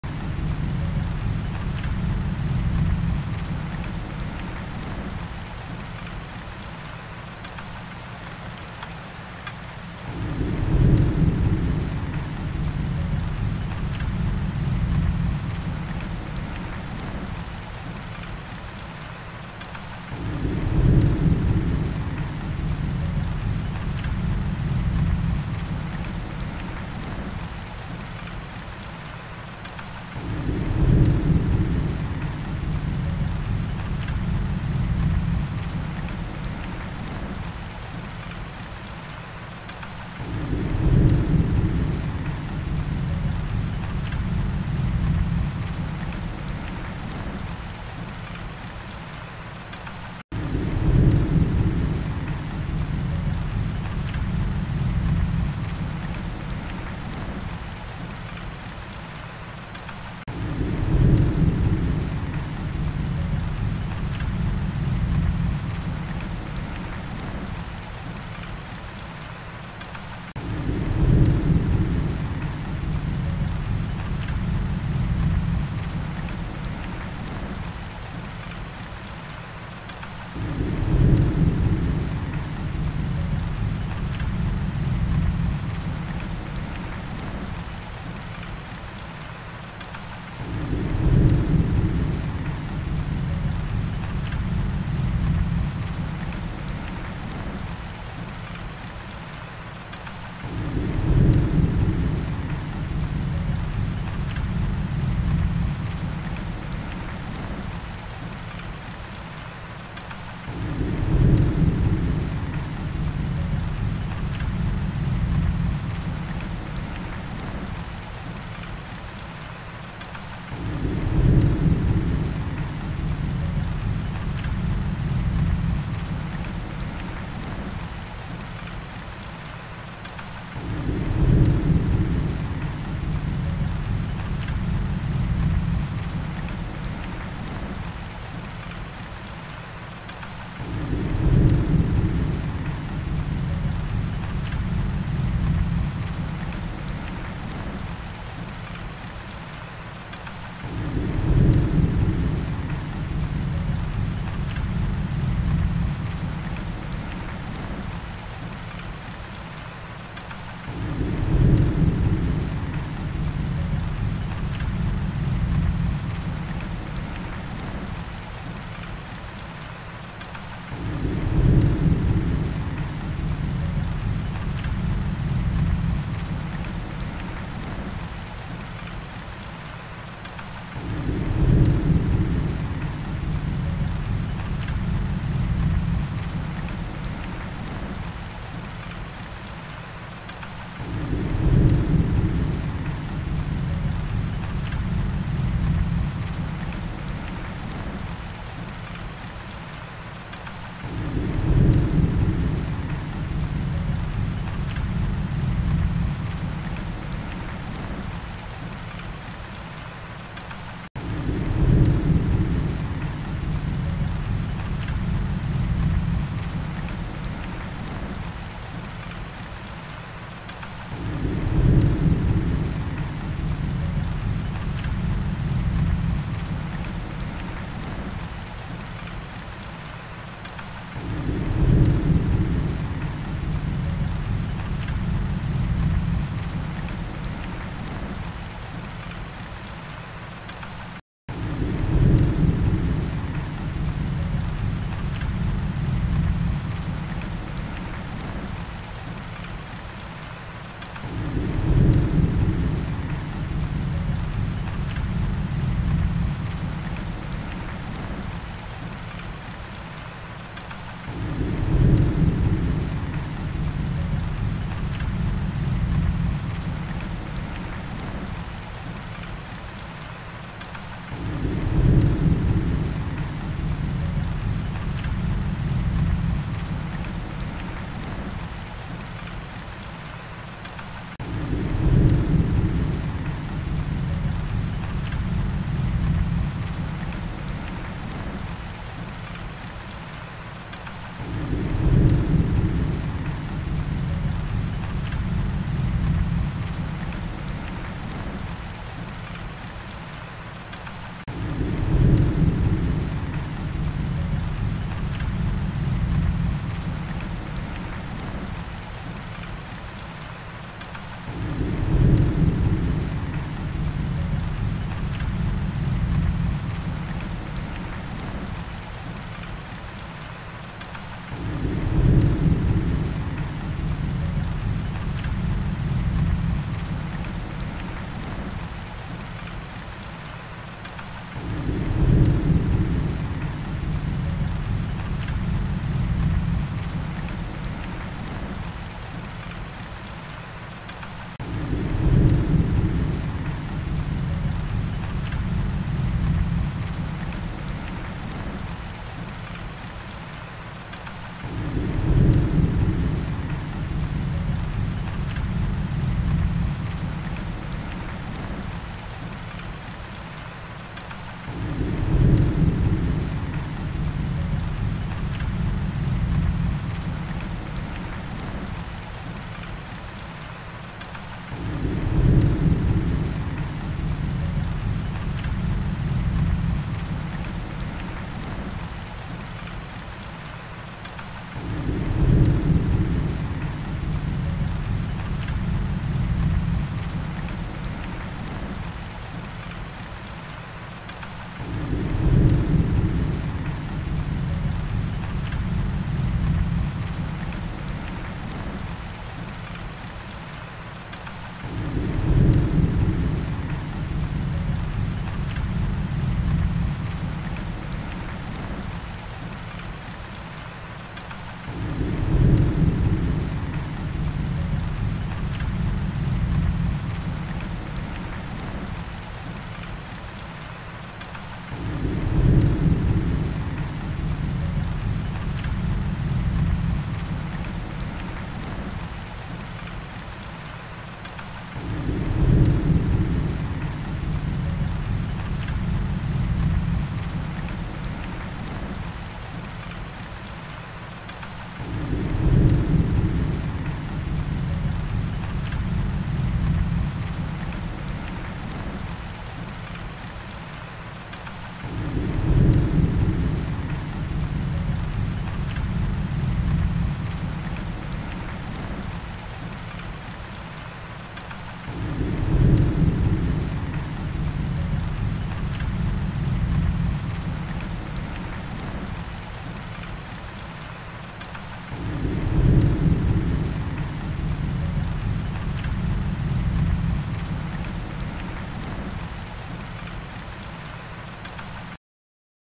Relaxing Rainstorm - Longer Version - 1212 Energy
I absolutely LOVE the sound of rain falling. As a kid (Gen X here!), my Mom would buy CDs with rain sounds. Usually they would have some funky acoustic electronic music playing. Or birds. I love birds, but do birds sing when it's raining? I don't think so.

Anyway...please enjoy this video of a relaxing rainstorm, the storm I always wanted but couldn't seem to find on a CD. I hope you enjoy it too!